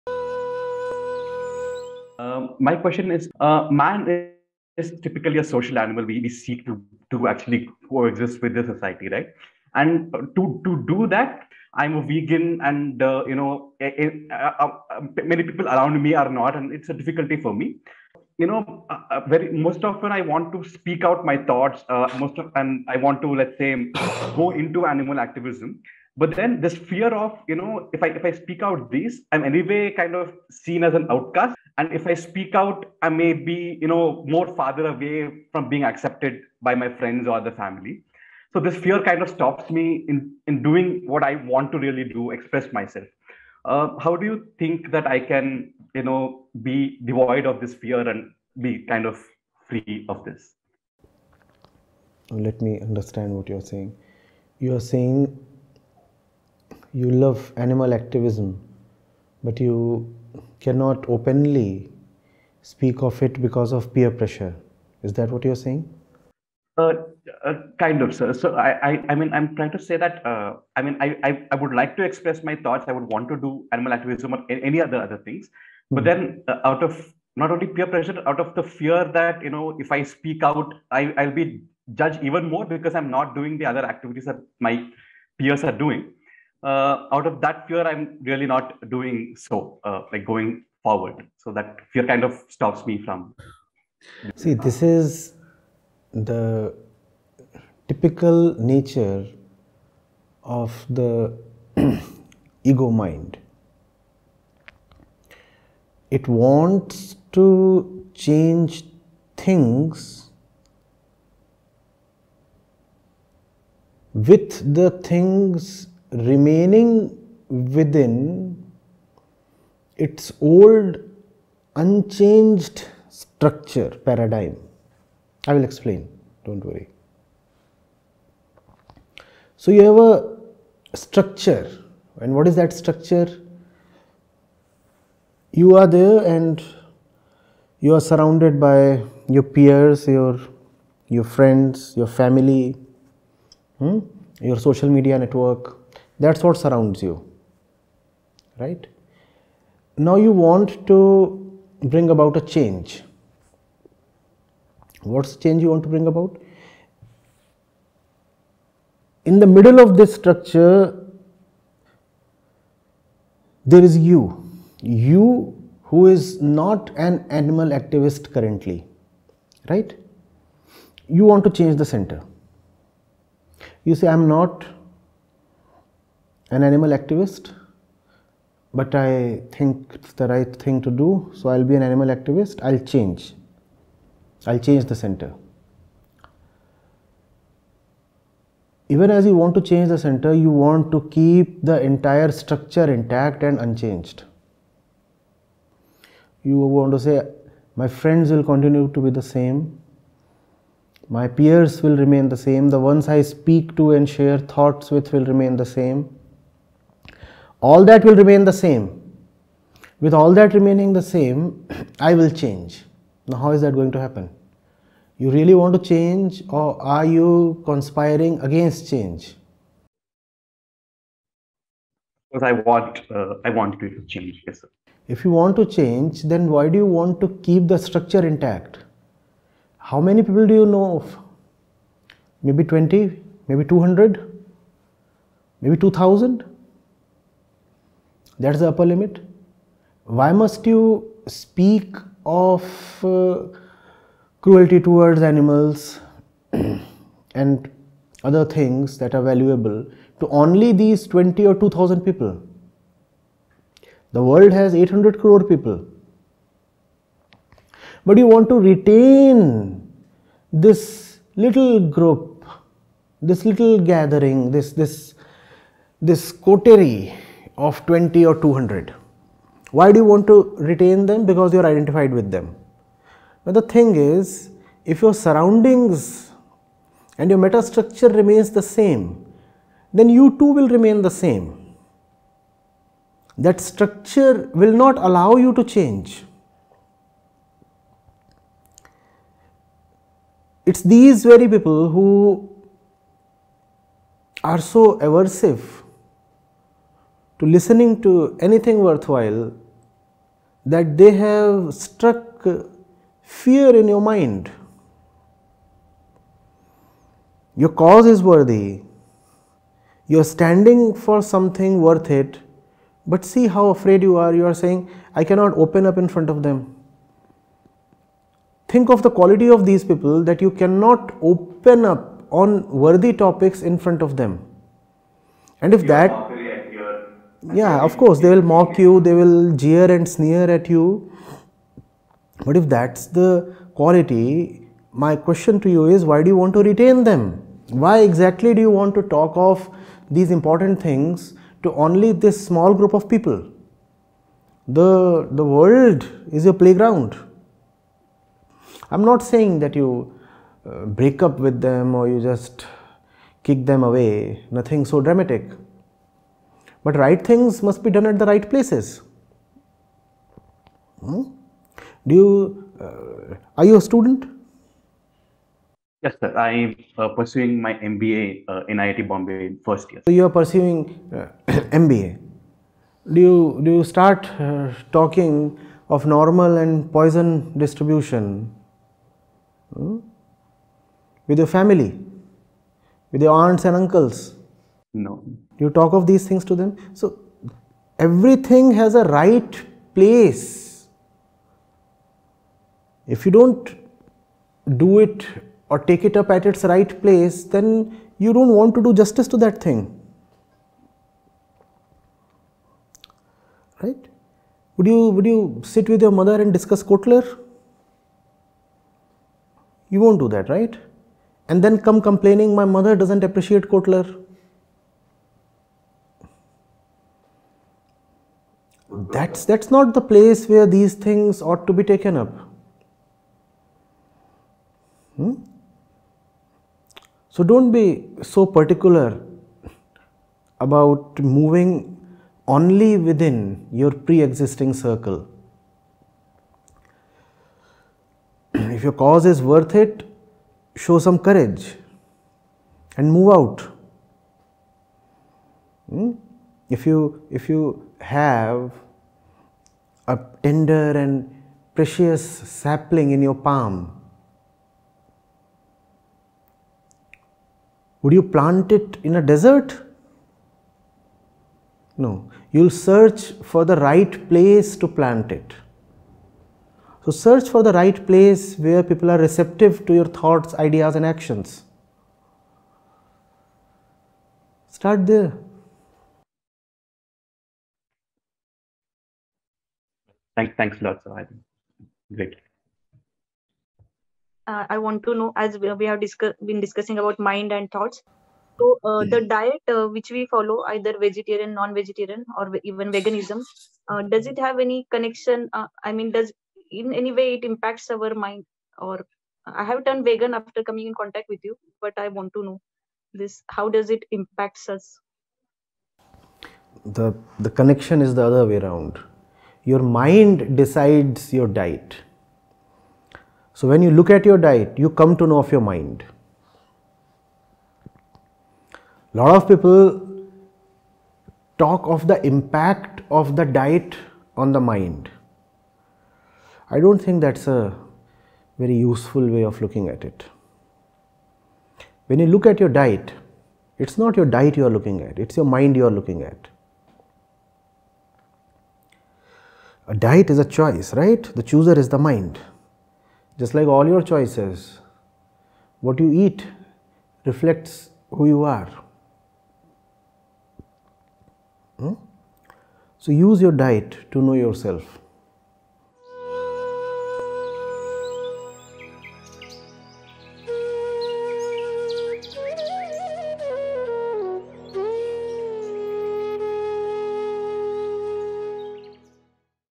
Suffering under peer pressure? || Acharya Prashant, with IIT Bombay (2022)
Video Information: Shastra Kaumudi, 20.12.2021, Rishikesh, India

Context:
~ Why an IITian loses focus - and what's the purpose of life
~ Why there is so much suffering in life?
~ Do we suffer by our own choice?
~ How to avoid pain and suffering?